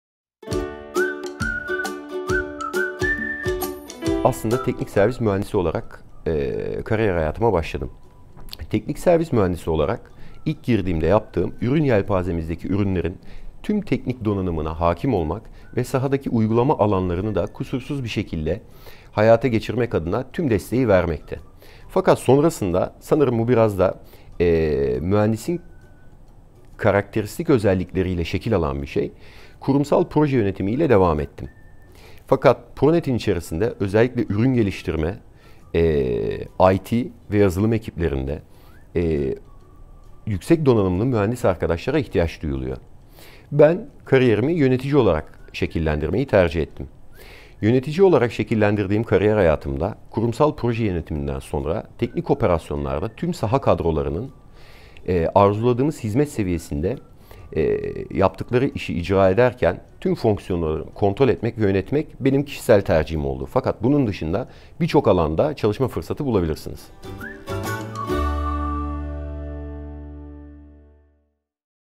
Pronet'te Mühendislik alanında nasıl kariyer fırsatları var?
Pronet Teknik Operasyonlar Direktörü Cafer Sefer Anlatıyor!

Pronet'te bir mühendis olarak çalışmak için nasıl özellikler gerekir?